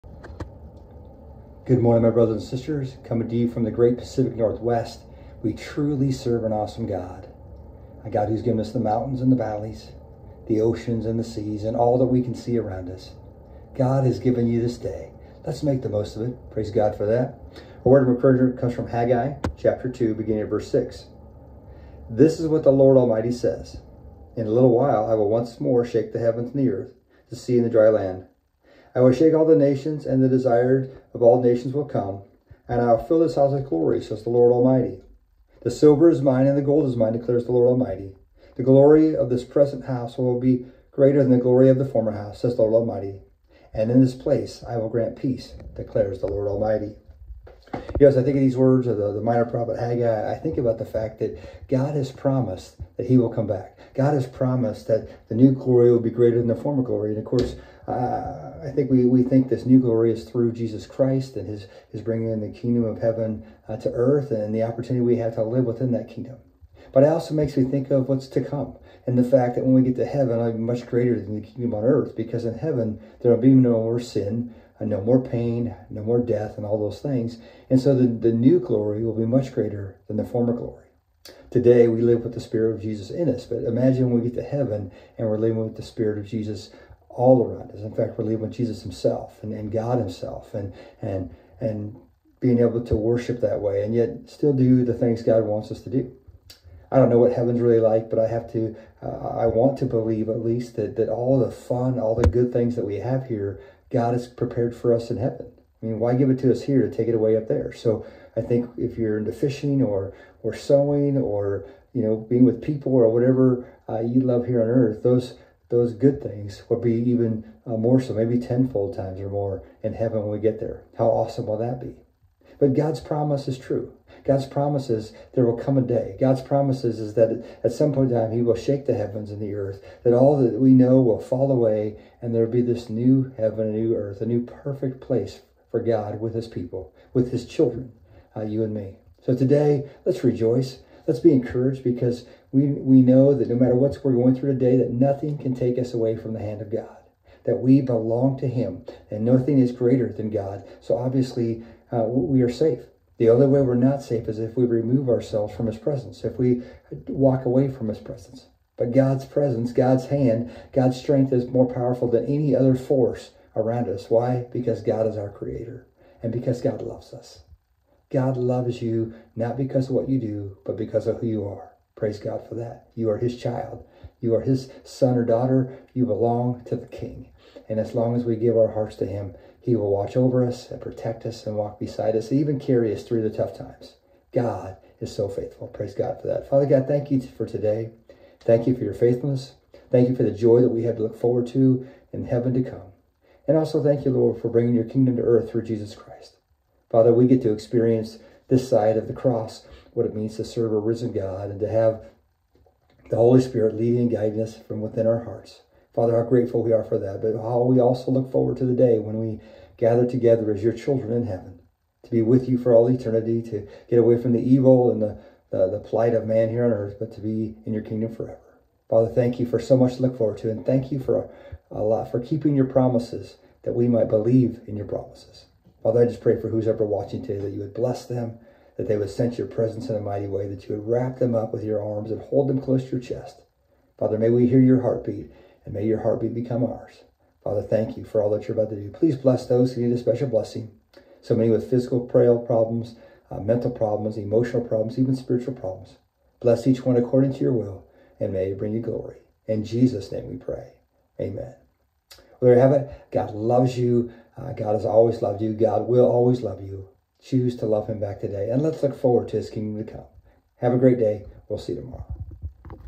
Daily Word of Encouragement
A daily word of encouragement using Haggai 2:6-9